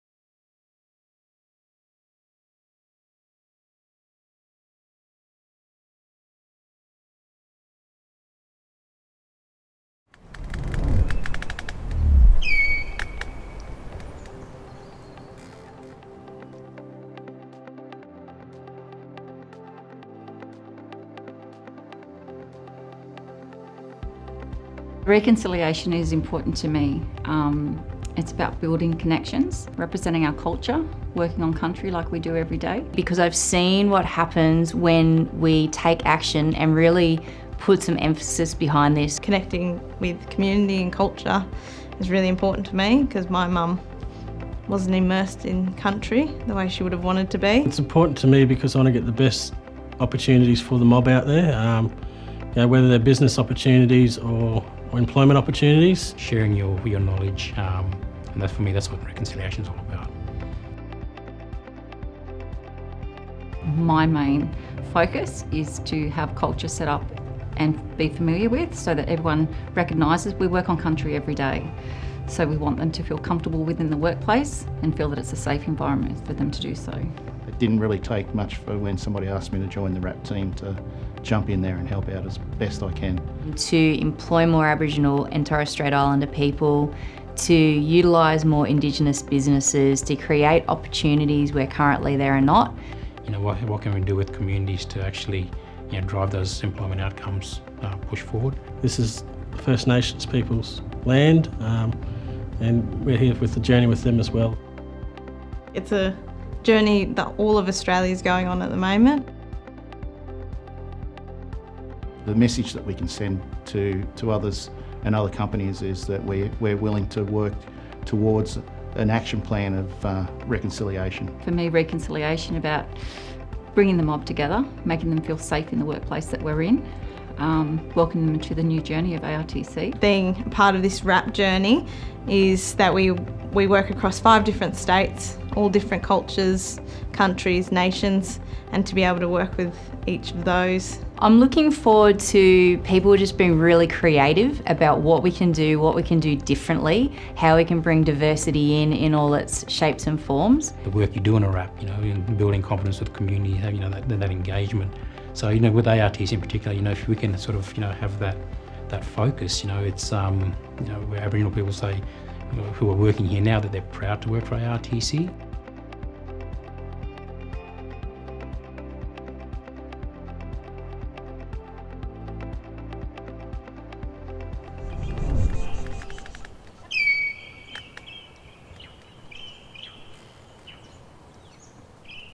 Reconciliation at ARTC
Members of our Reconciliation Action Plan Team shared their perspectives on reconciliation at the RAP Launch on Wonnarua Country in March 2022.